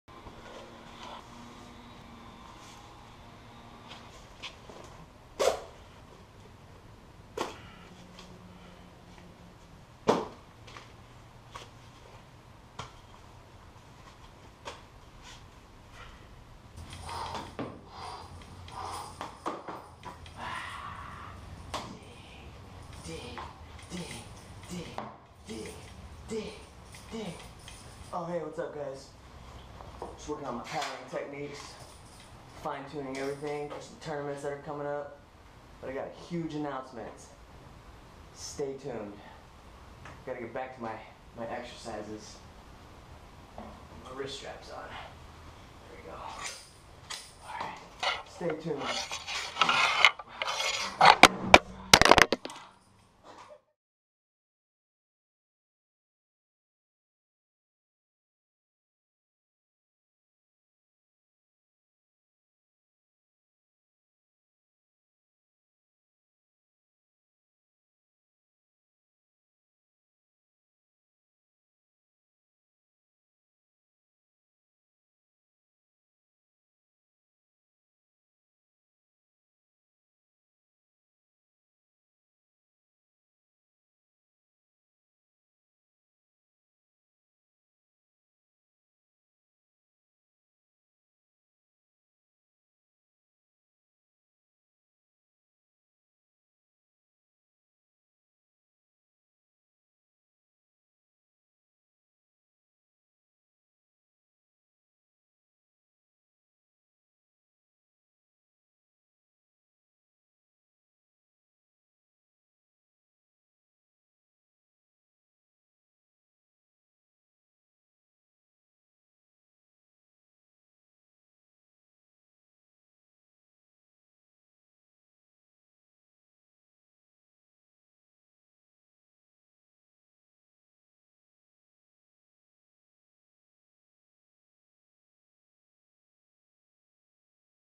"DIG PULL DIG PULL" (KAYAK EXERCISING)
DIG PULL DIG PULL to become a better kayak angler. In this fishing video skit I teach you how to become a better kayak angler.